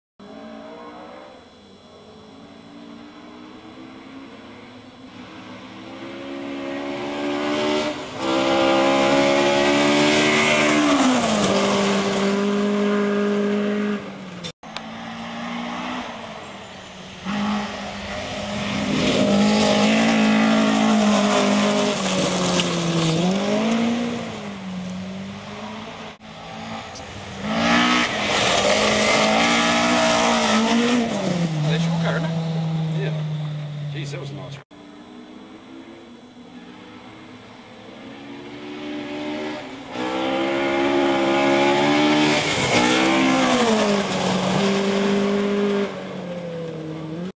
BLUE DX NZ MARK PARSONS 04 JAMES ALLEN MARAMARUA RALLY
Lent my car to Mark this weekend due to him having a dud run on the Rally Of The Far North a couple of months earlier.
This time car ran 100% and all was good in the forest.
See the incar on another clip.
BLUE DX NZ MARK PARSONS 04 JAMES ALLEN INCAR MARAMARUA